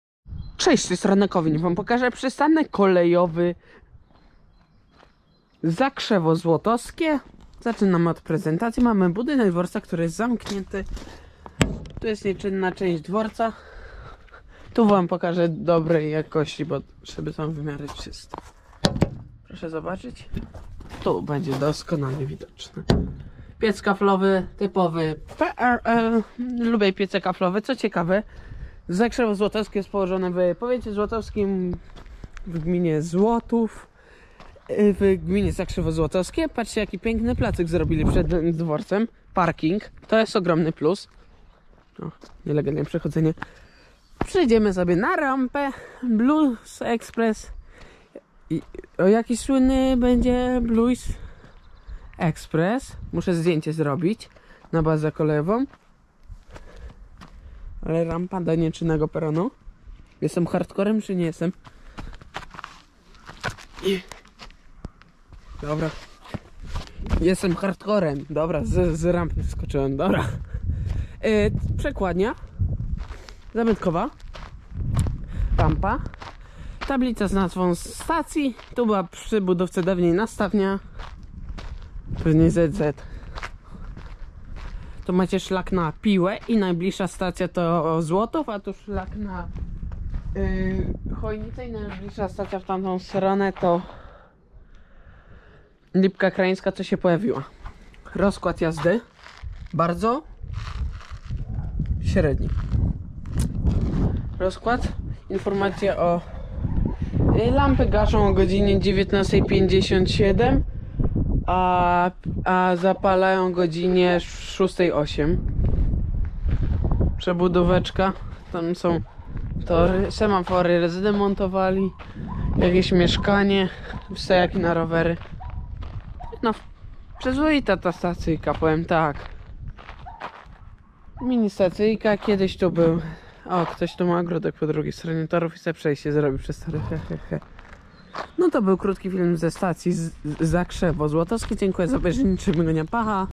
Przystanek kolejowy Zakrzewo Złotowskie -"Polowanie na pociągi"
Hej!
Dziś przystanek kolejowy Zakrzewo Złotowskie!

Dane Zakrzewa:
Województwo: wielkopolskie
Powiat: złotowski
Gmina: Zakrzewo
Liczba ludności : 1620

Dane przystanku
Perony: 1
Krawędzie peronowe: 1
Kasy: zlikwidowane